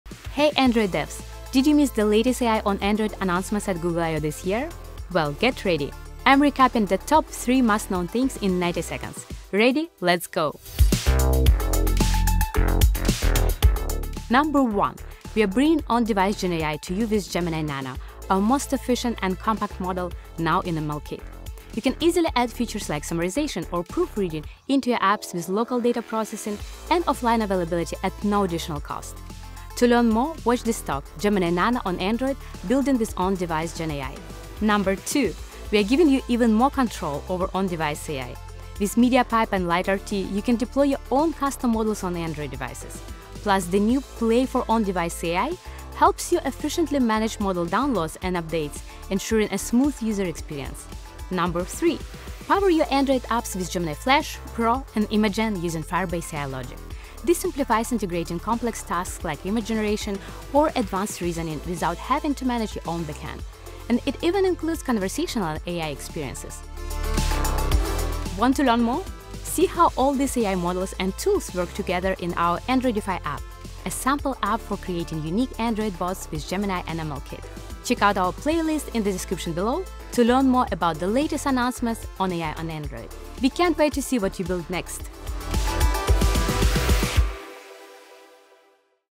Top 3 AI on Android announcements at Google I/O ‘25
Build more intelligent apps on Android! The latest from Google I/O 2025 lets you enhance on-device tasks with Gemini Nano and integrate the power of Gemini Flash and Pro via the Firebase AI Logic.

Products Mentioned: Android